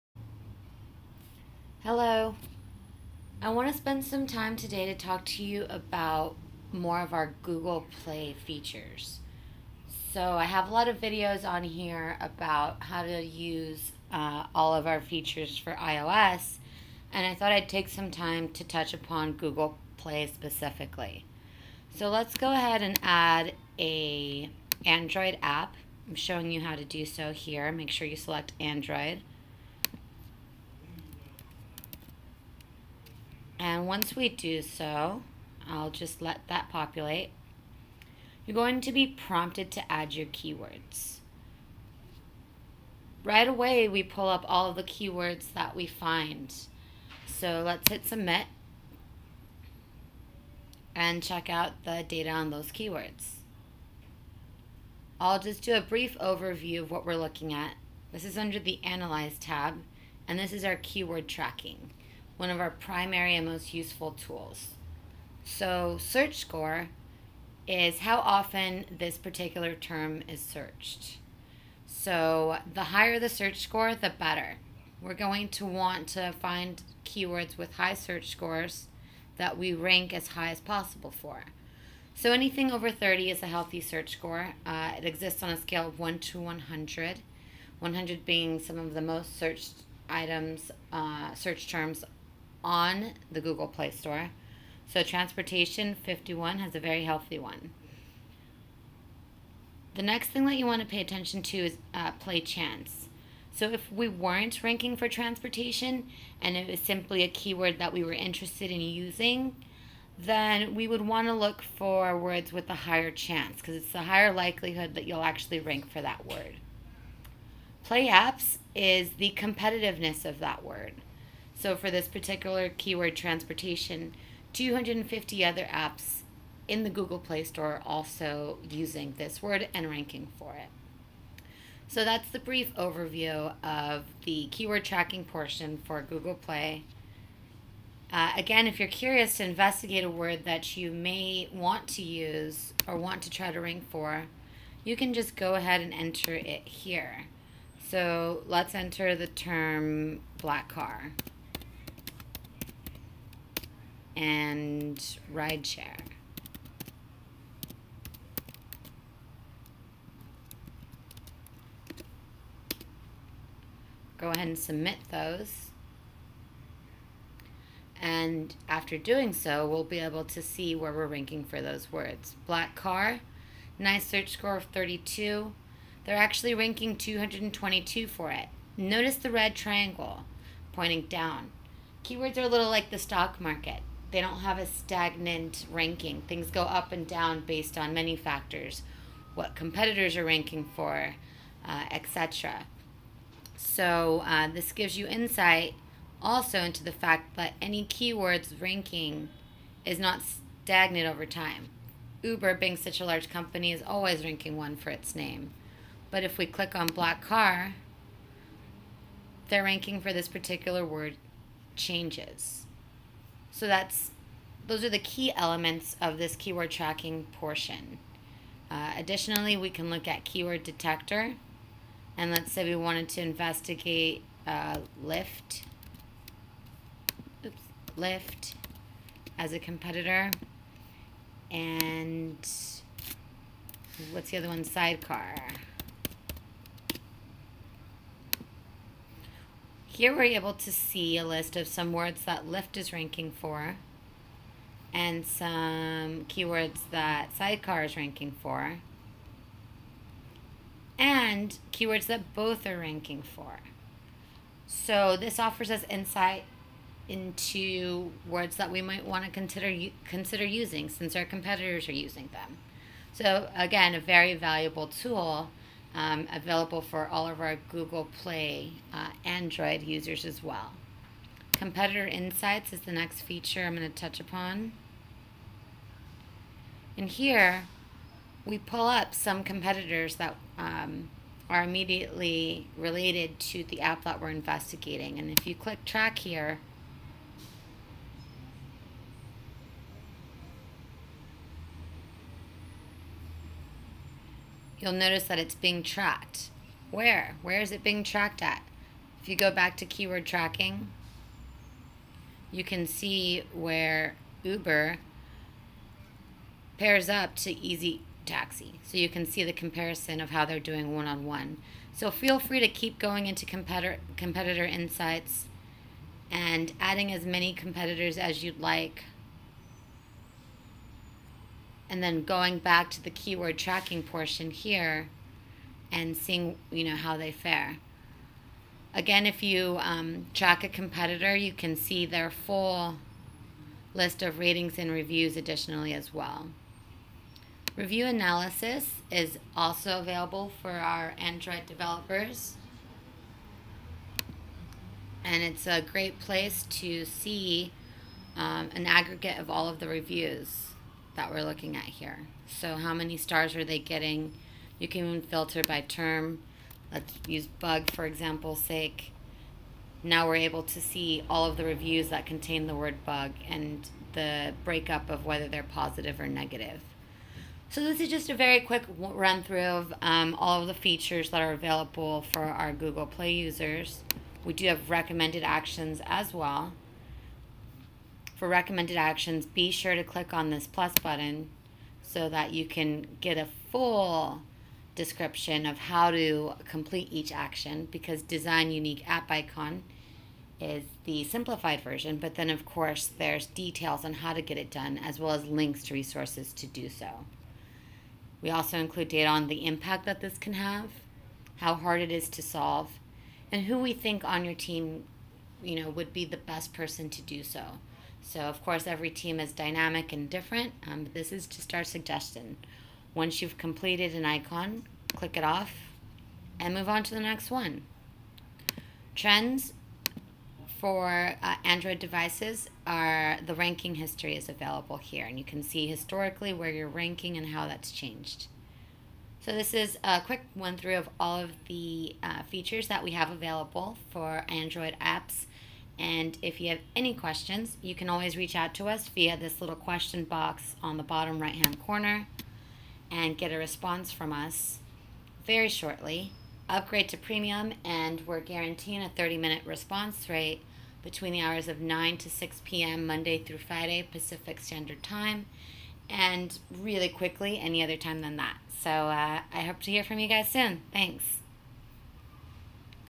Mobile Action-App Store Optimization- App Store Intelligence Tool-Google Play Dashboard Overview
Find out how to best utilize our features to improve your Android app's visibility. Remember, repeat desired keywords within your description 4-5 times as a best practice to rank for desired keyword.